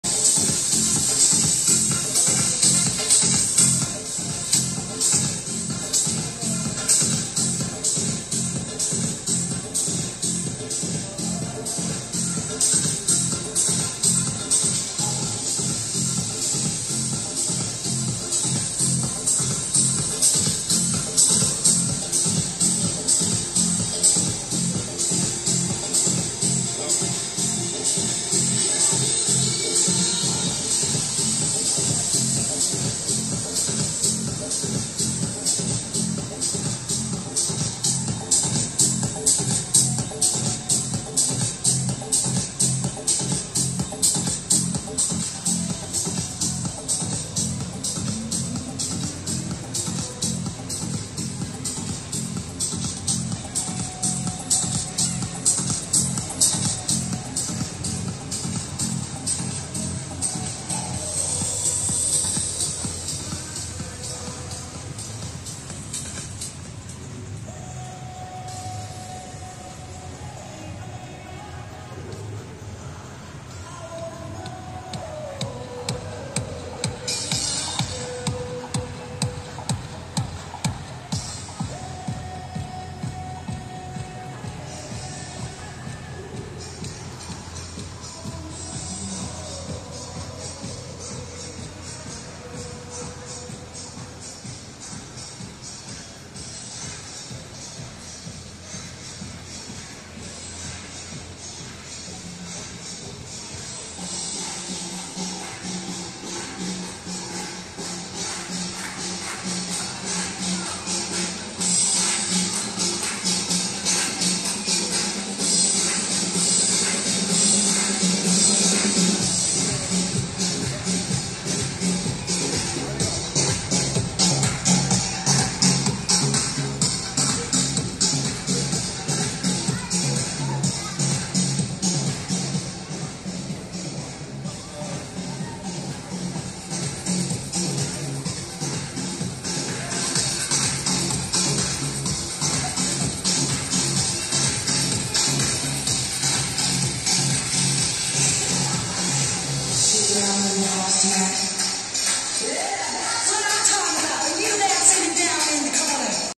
A Headless man in UAE For people amusement at Al Hamra Mall
AlHamra mall